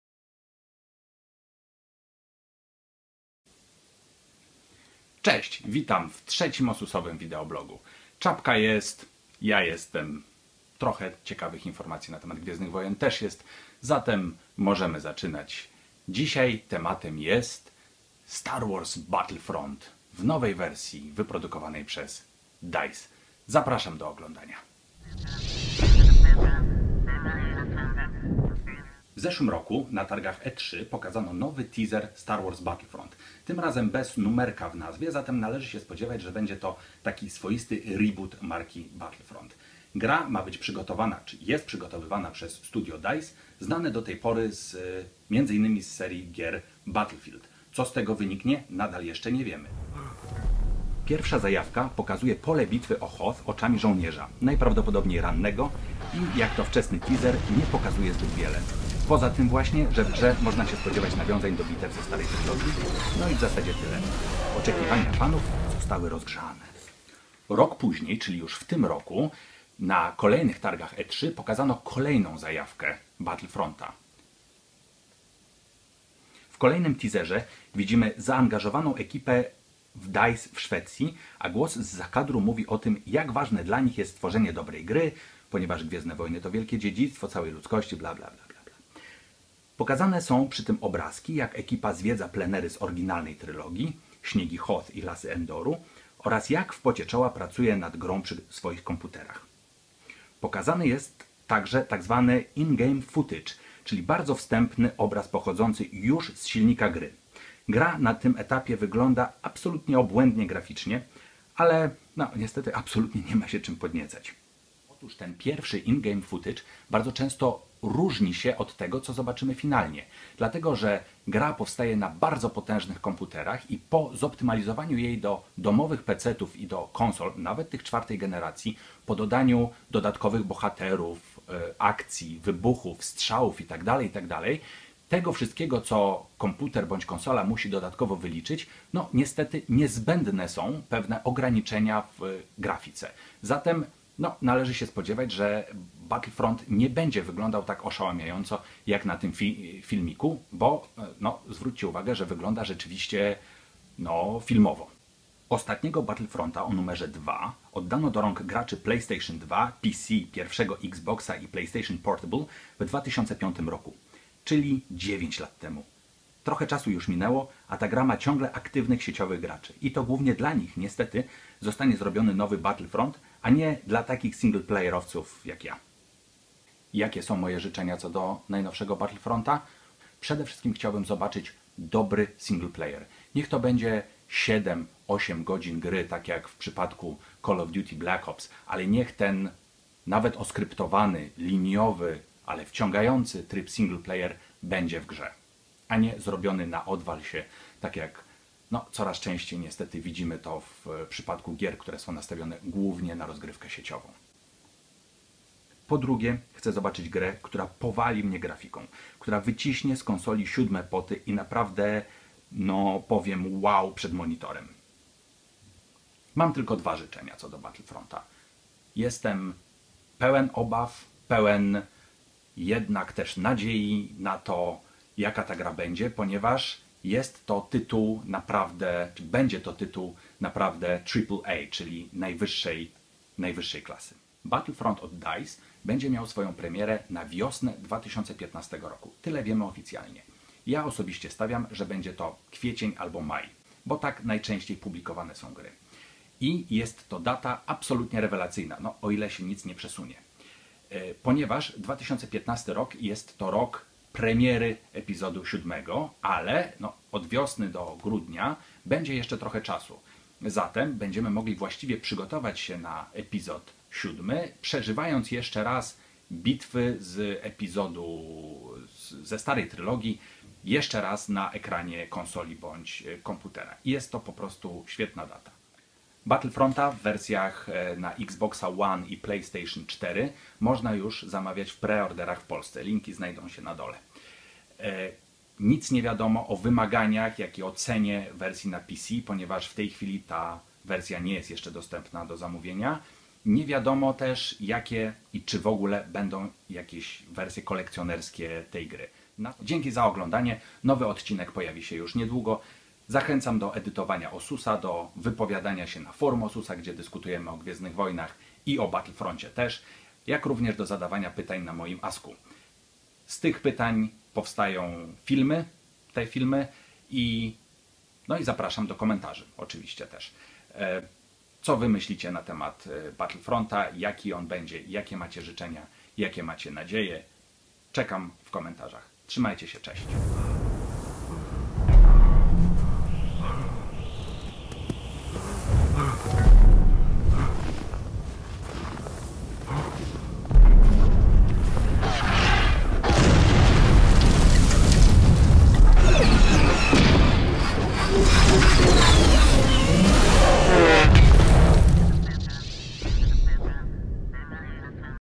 Ossus Vlog #3 - Battlefront od DICE
DICE, czyli firma znana z produkcji gier z serii Battlefield jest odpowiedzialna za nową odsłonę gry Star Wars Battlefront. Wydawcą będzie EA. Co wiemy? Jakie są oczekiwania? Tego dowiecie się w tym filmiku!

- mój AskFM - zadawajcie pytania i dajcie materiał na nowe Vlogi!

- Battlefront na PS4 za 249 PLN
- Battlefront na XB1 za 267 PLN